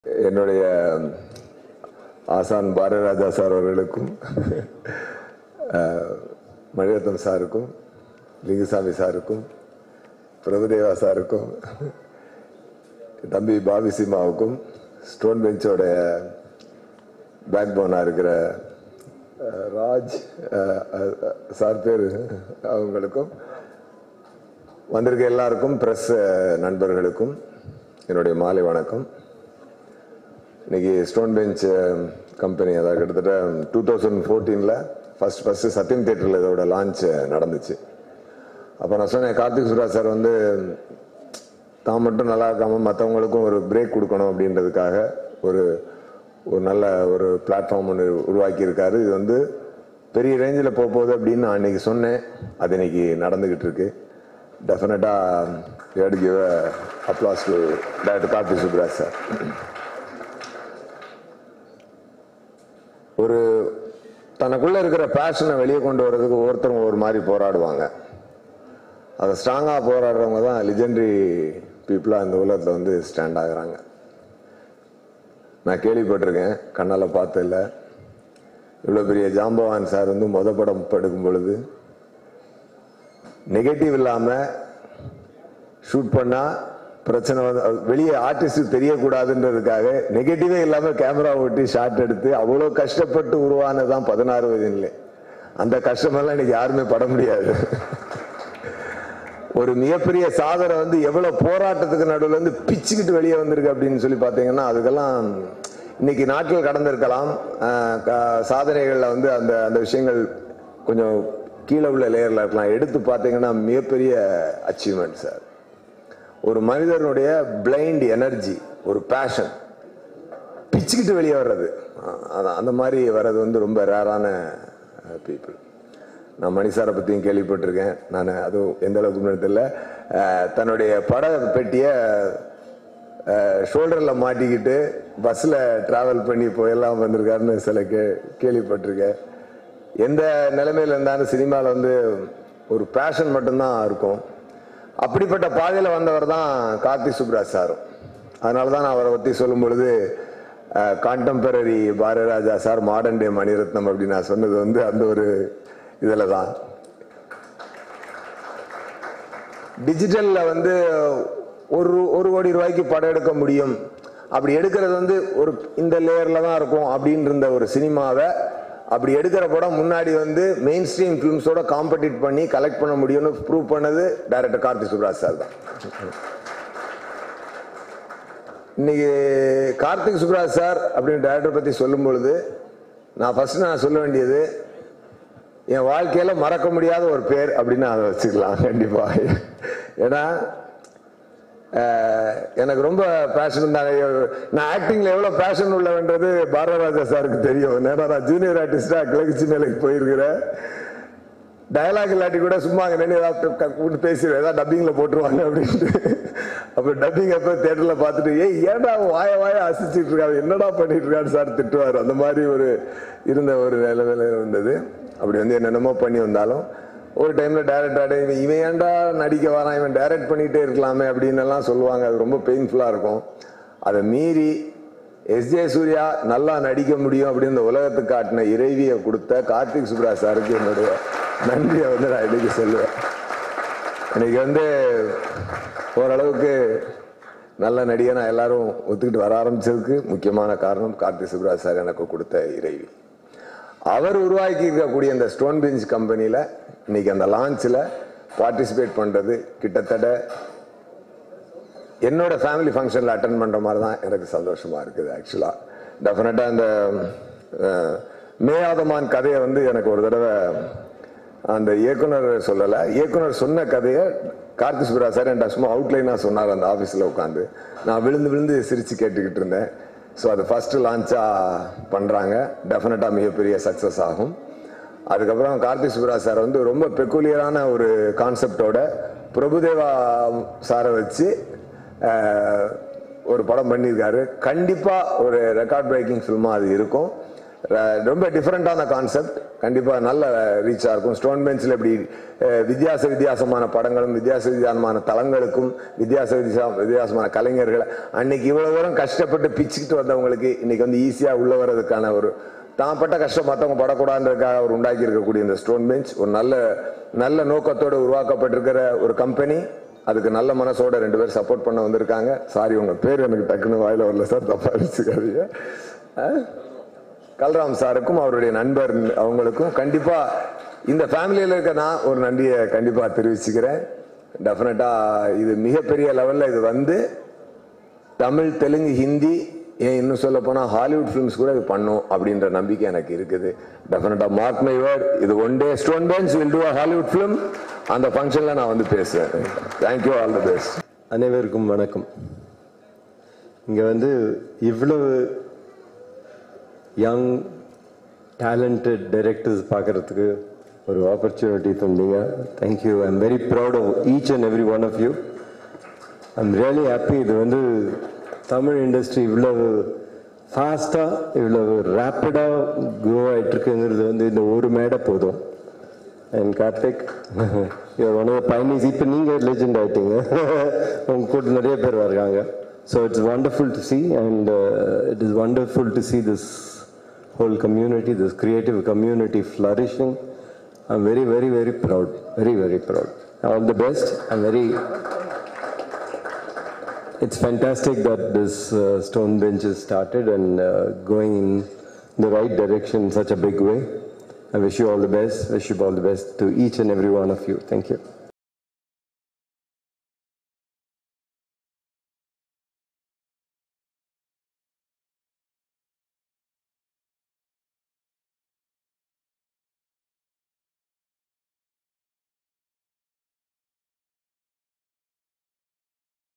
Manirathnam Speech - என் வாழ்நாளில் மறக்க முடியாத பெயர் கார்த்திக் சுப்ராஜ் - Tamil Cinema News
Latest Tamil Cinema News